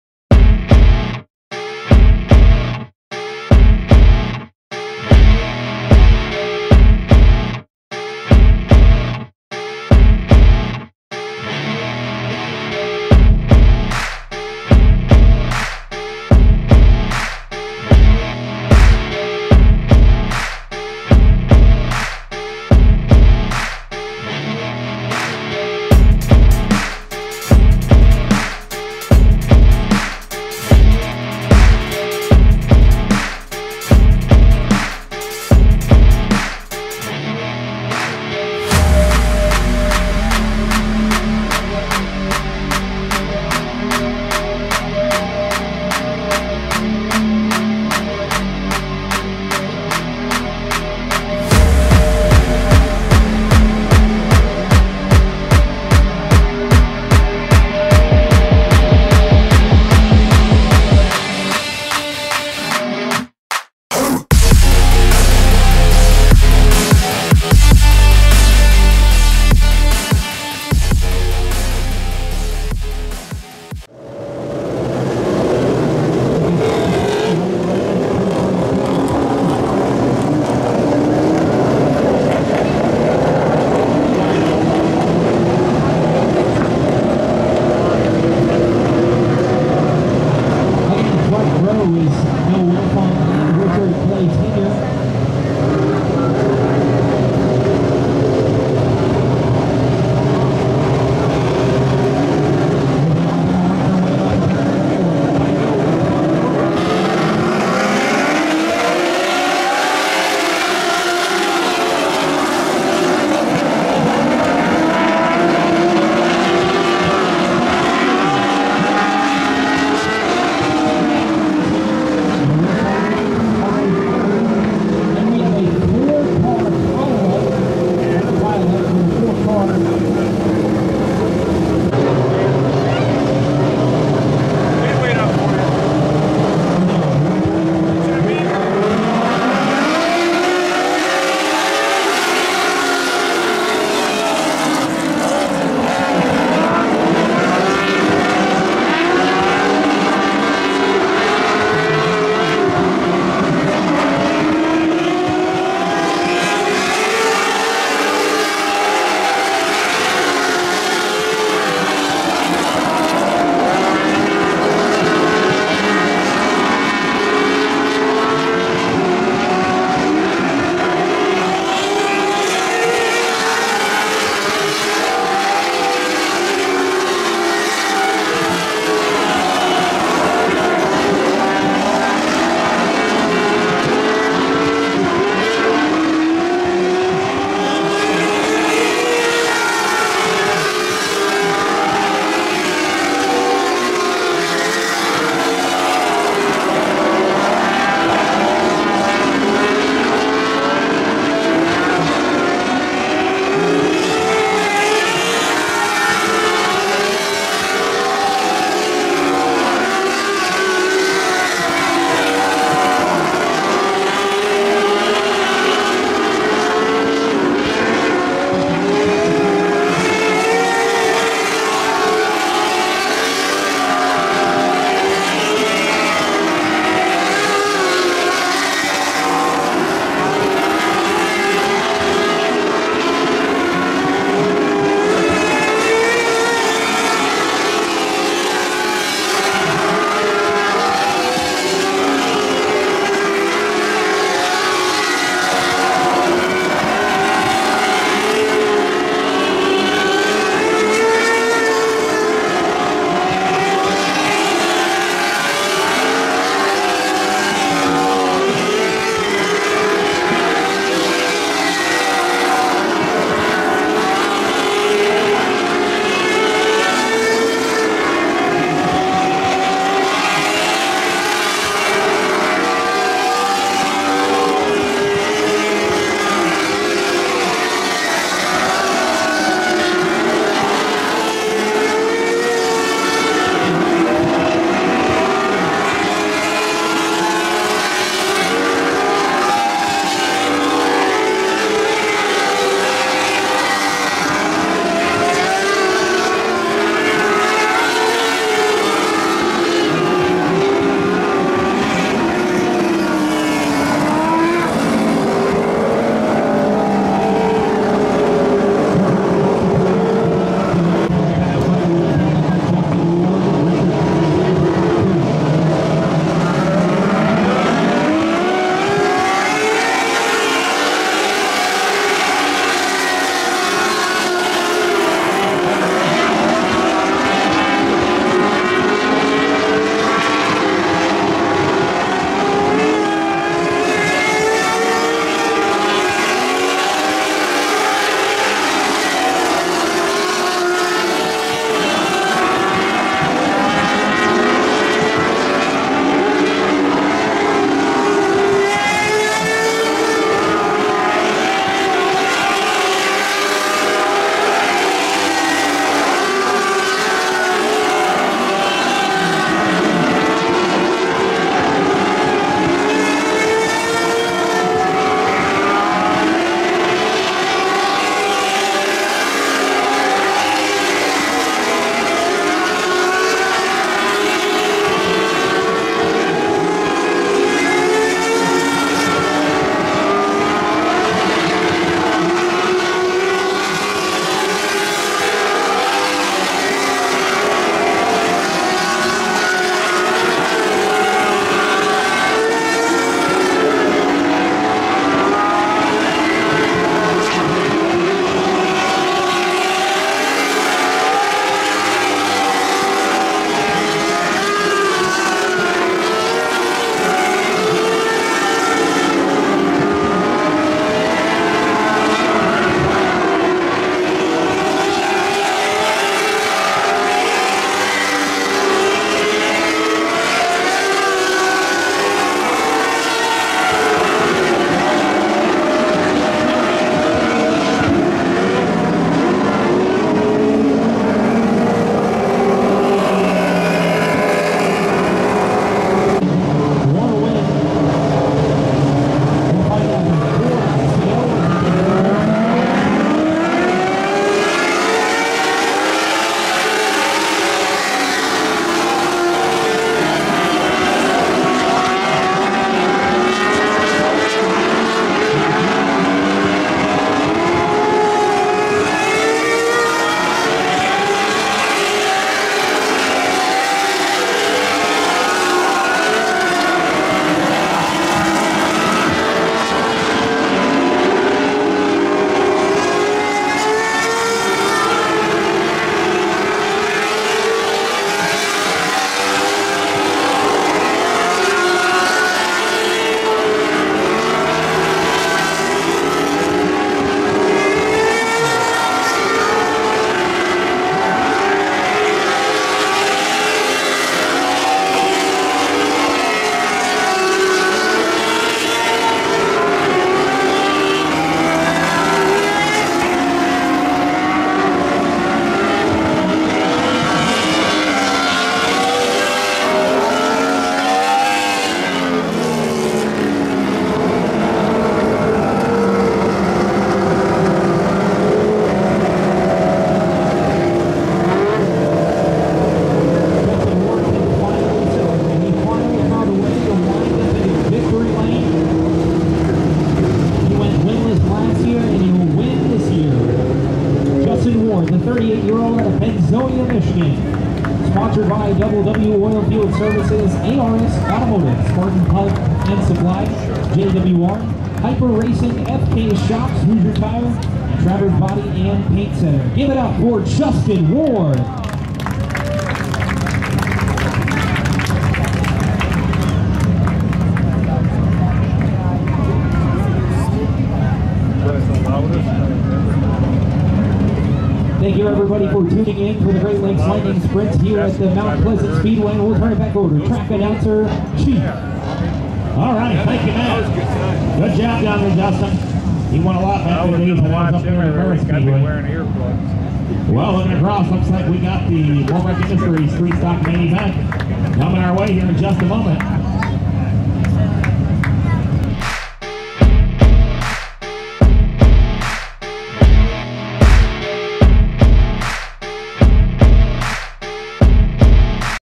Great Lakes Lightning Sprints A-Feature at Mount Pleasant Speedway, Michigan on 07-07-2023!!
United Engine Machine A-Feature 1 (20 Laps): 7-Ricky Droke, 26-Richard Clay Sr, 8-Justin Ward, 21-Jacob Sabaj, 16-Noah Wilfong, 76-Dalton Pipgras, 155-Alex Cogley, 55-Matt Cogley, 40C-Chad Colley, 48-Jake Erickson, 101-Chuck Erickson, 54-Bud Schrader, 29-Arlie Cooper Jr, 45-Bobby Miller, 34M-Sherman Napeir, (DNF) 25-Paul King, (DNF) 19-Avery Neal, (DNF) 116-Kevin King, (DNS) 273-Jerry Napeir